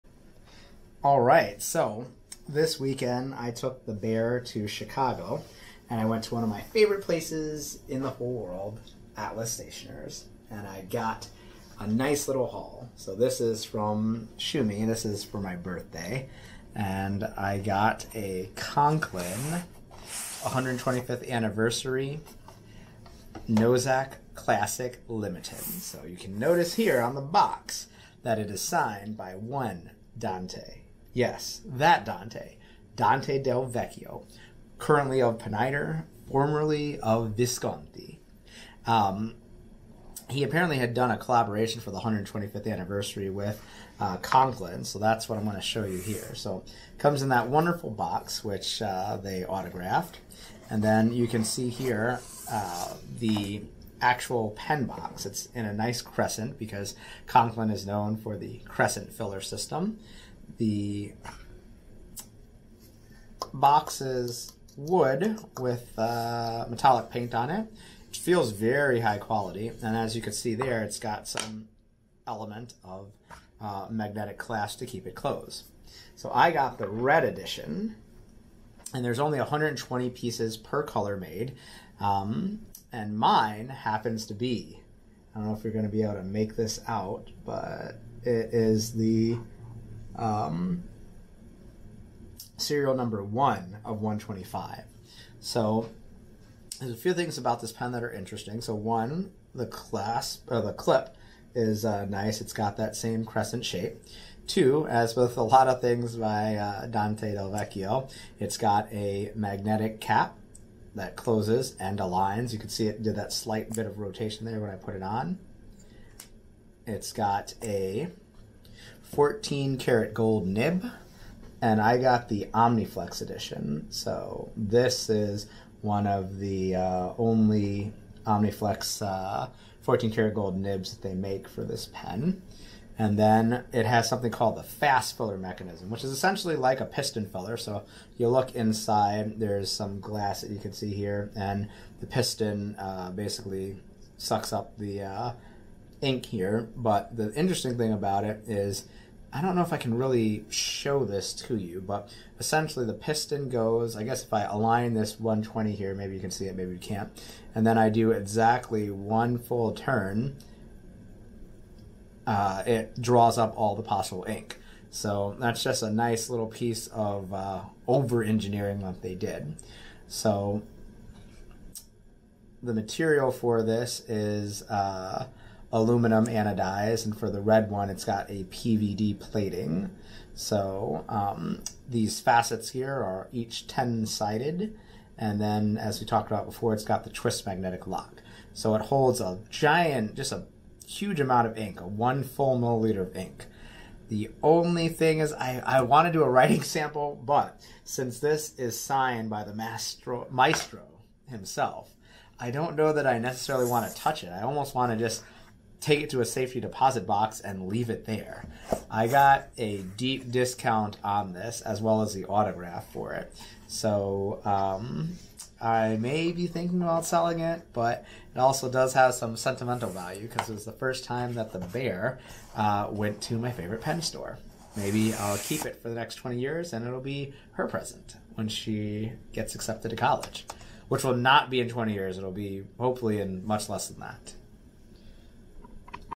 Unboxing Conklin Nozac Limited 125th Anniversary Edition
Bought a limited edition Conklin from Atlas Stationers. Box was signed by Dante Del Vecchio himself at the Holiday Takeover event.